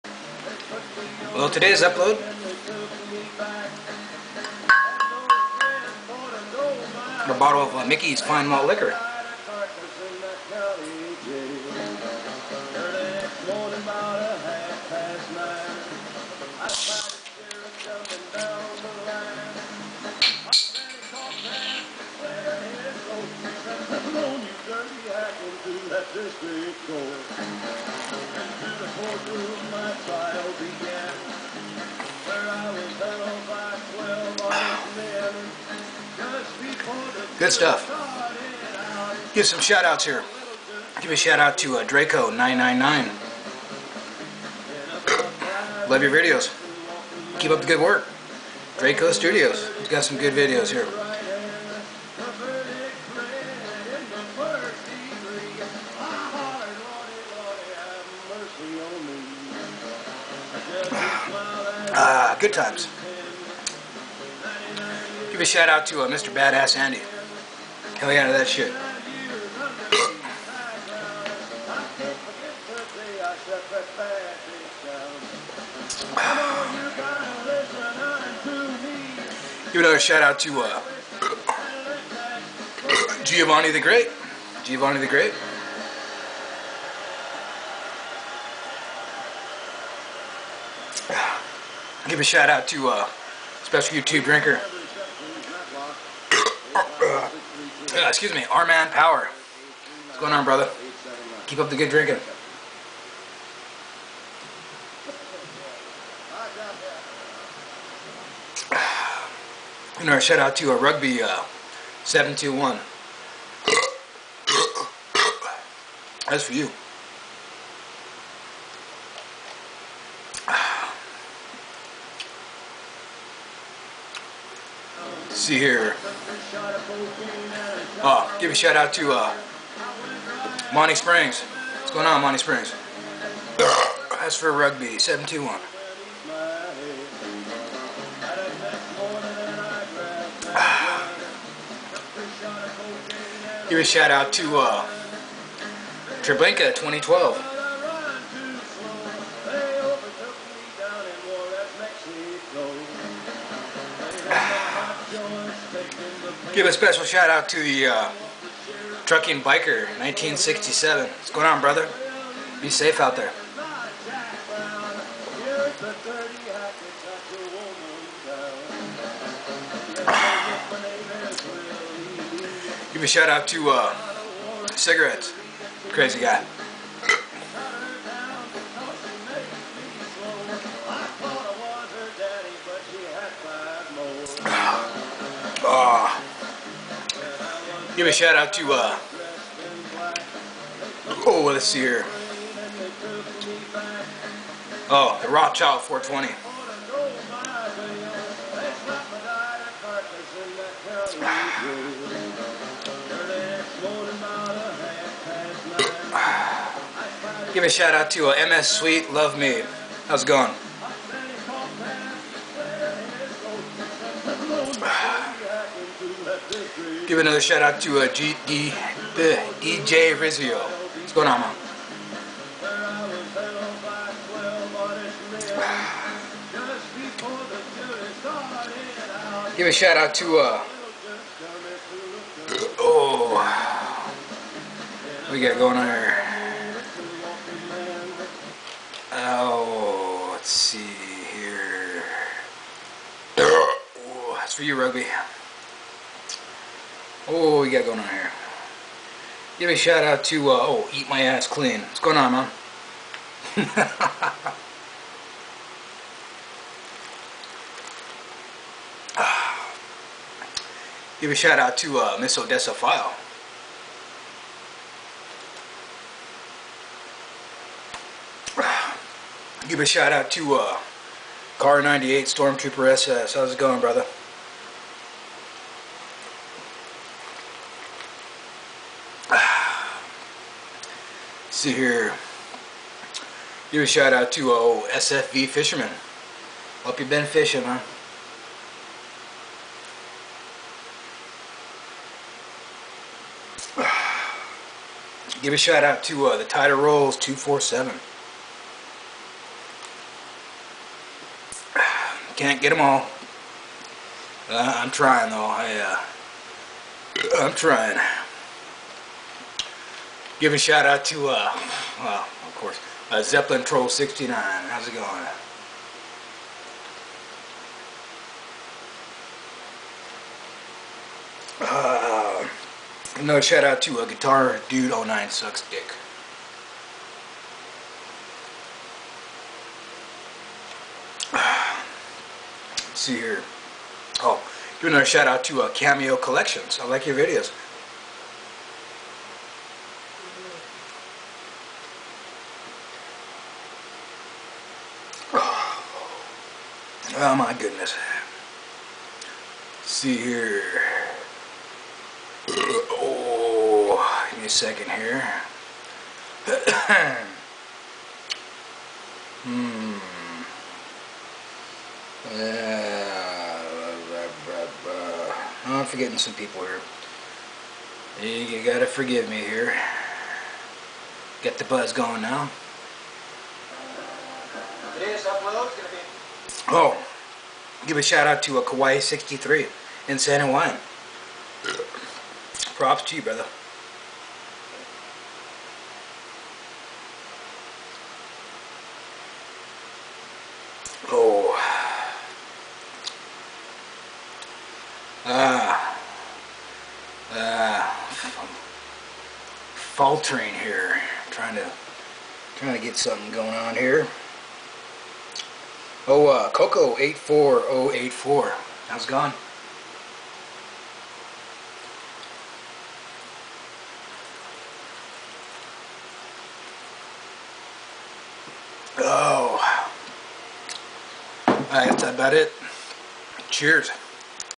40OZ OF MICKEYS WITH SOME SHOUTS PART 2
Had sort of of BUZZ after slamming a STELLA ARTOS while making this upload.